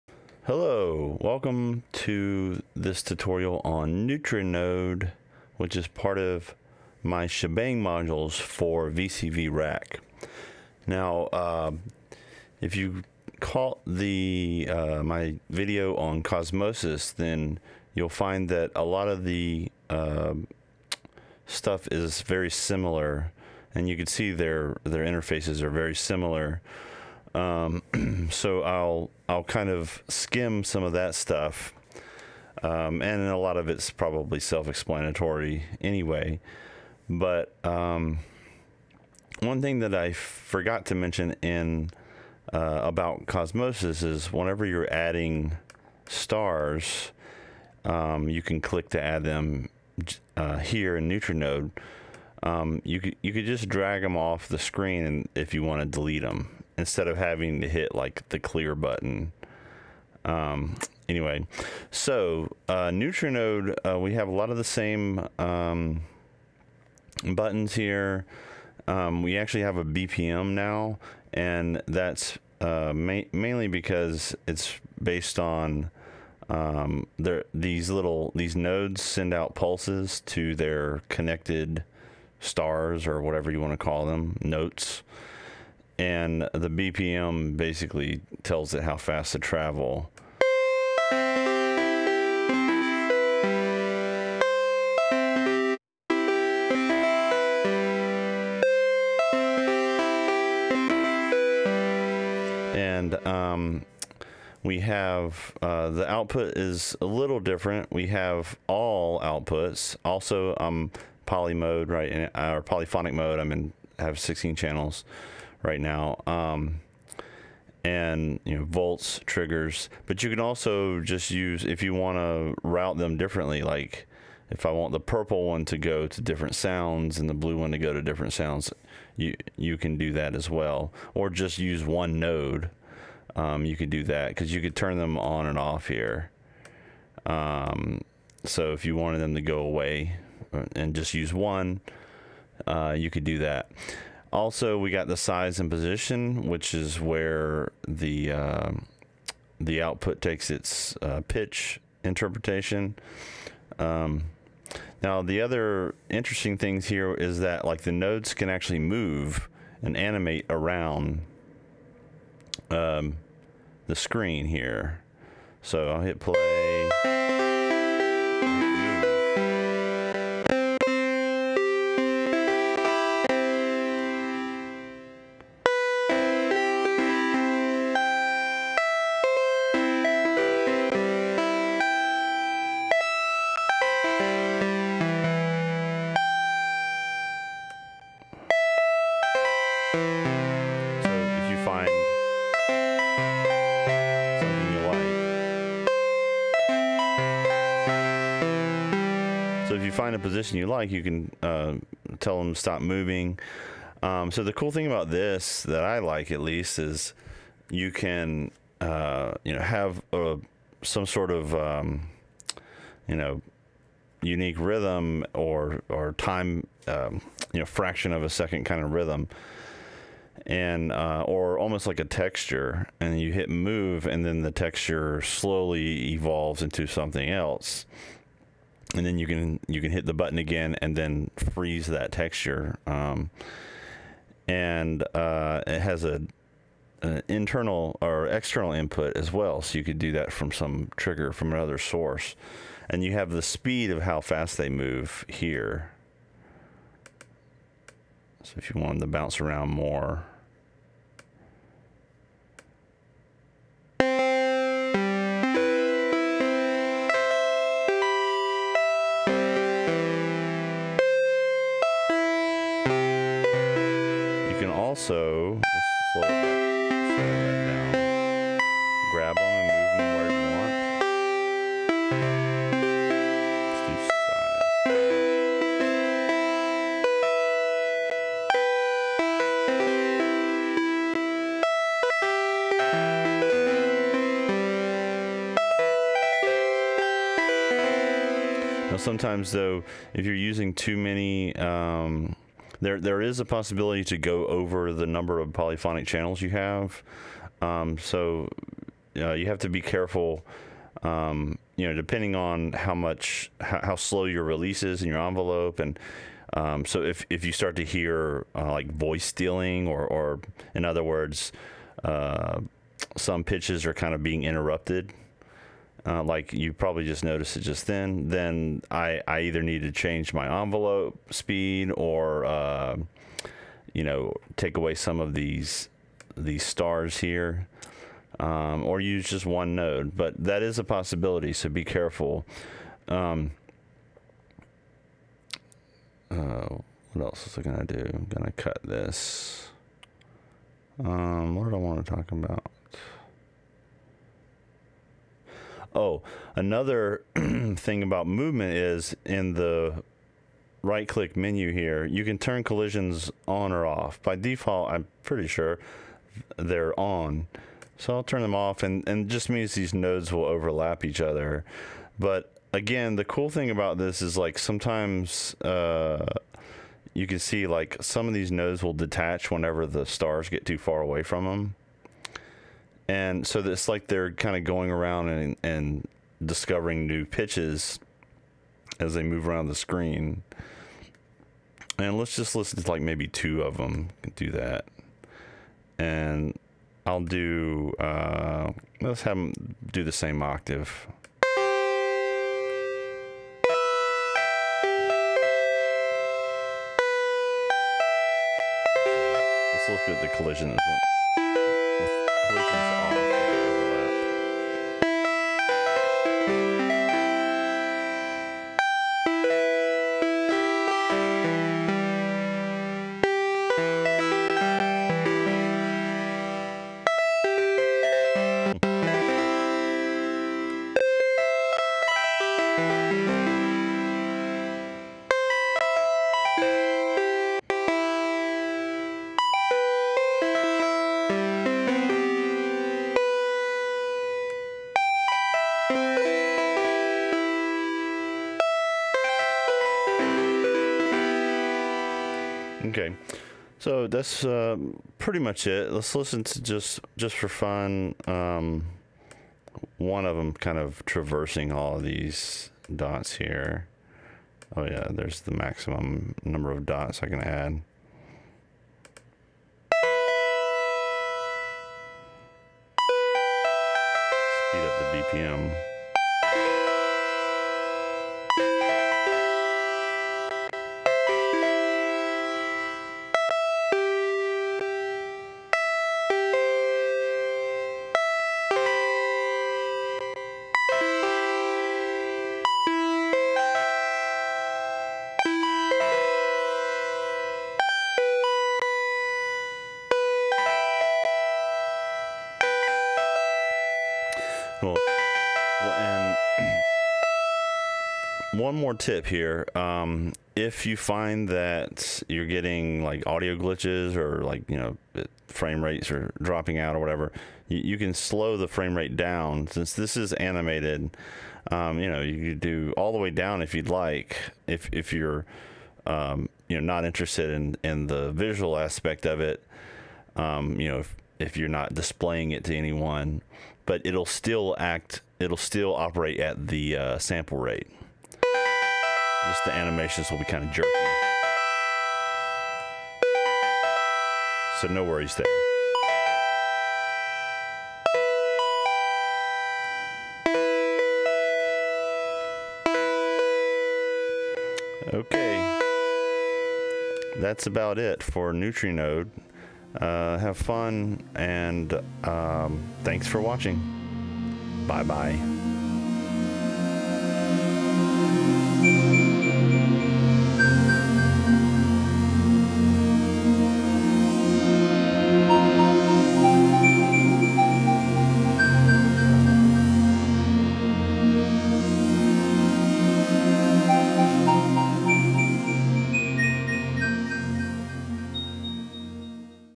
Neutrinode - Sha# Bang! Modules
Explanation of the Neutrinode module for VCV Rack. I spend most of the time in this video explaining how the Polyrhythm mode works.

Support me!